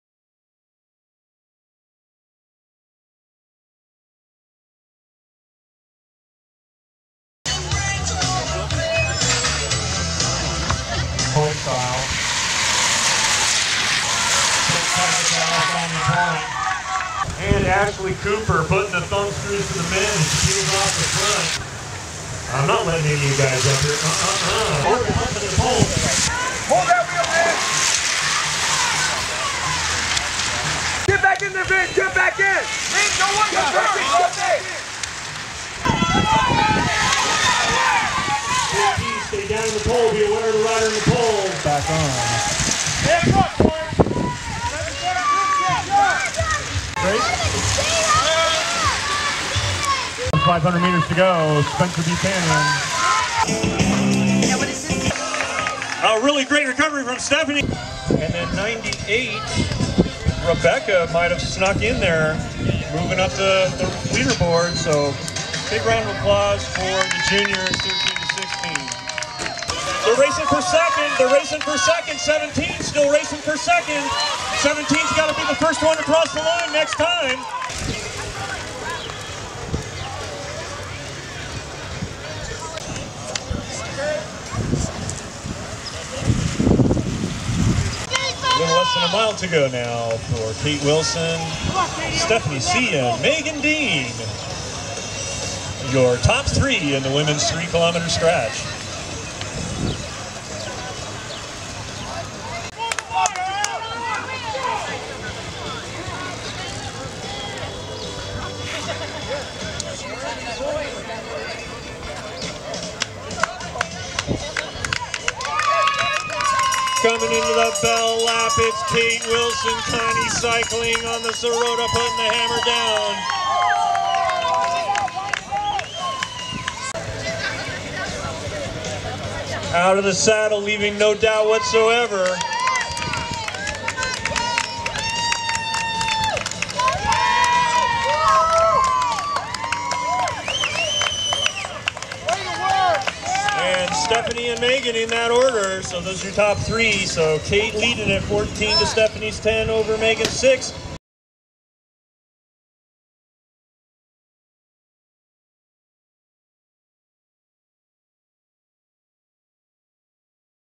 Bike Racing: Black Line Fever
Racing at the Encino Velodrome.